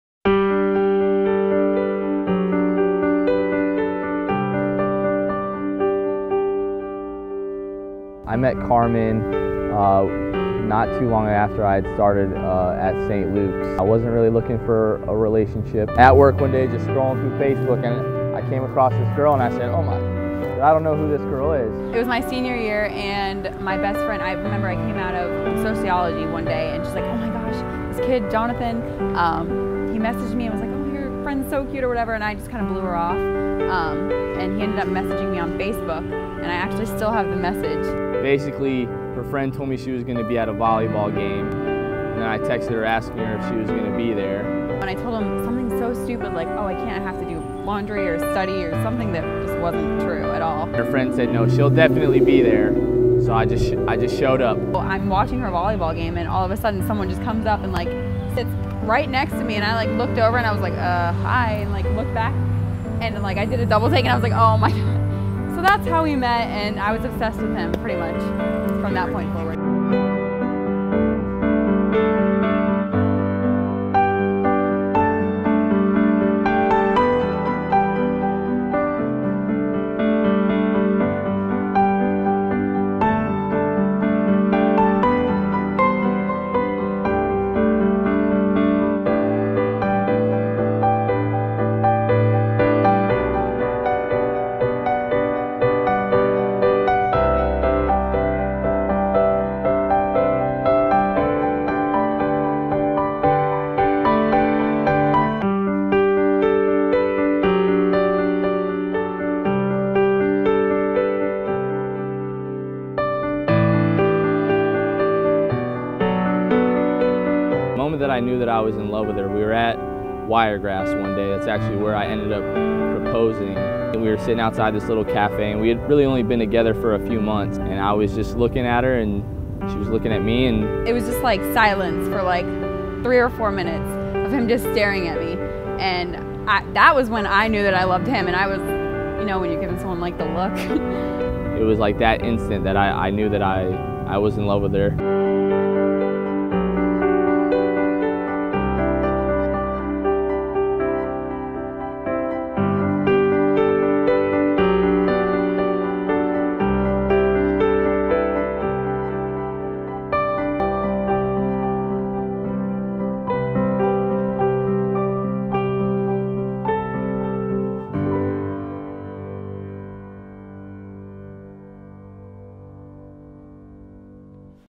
Carmen & Jon: Wedding Film at Polk County Courthouse in Bartow, Florida
Carmen and Jon had a small, yet gorgeous wedding. Cinema really focused on telling the couple's love story through interviews with the bride and groom. What a beautiful couple! The team over at Cinema wishes you endless years of happiness together.

If you would like to learn more about Cinema Wedding Videography services please